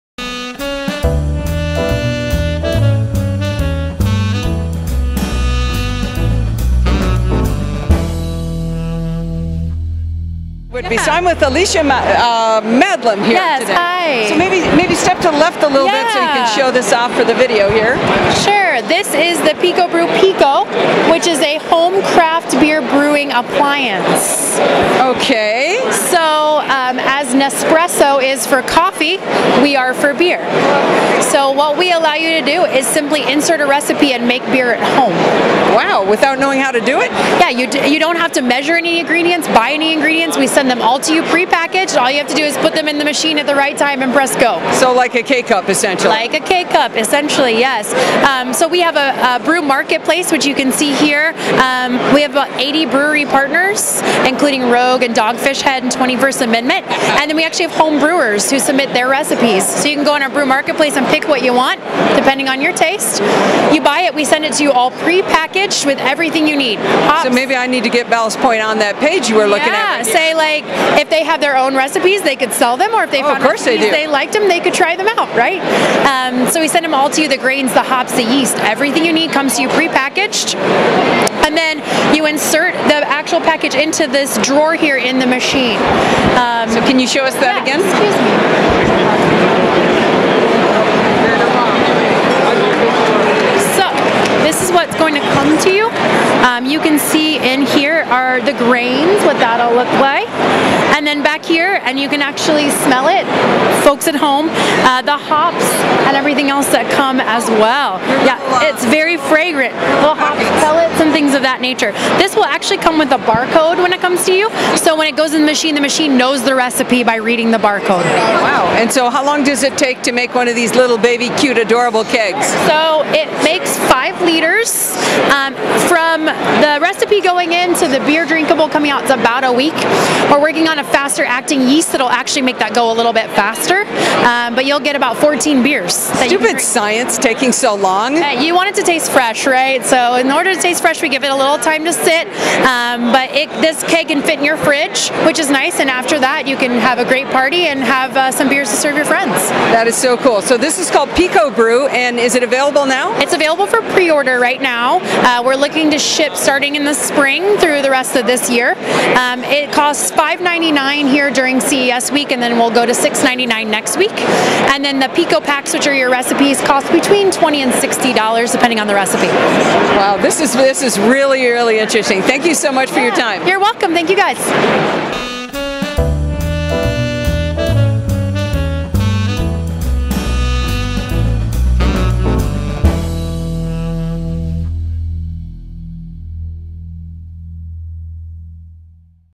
CES 2016: PicoBrew
Allison interviews Alyshia Madlem from PicoBrew about their new PicoBrew system. PicoBrew allows you to brew your own fresh, personalized craft beer using professional equipment and convenient, ready-to-brew PicoPak ingredient kits from the worldwide BrewMarketplace. The setting is the CES Unveiled show floor.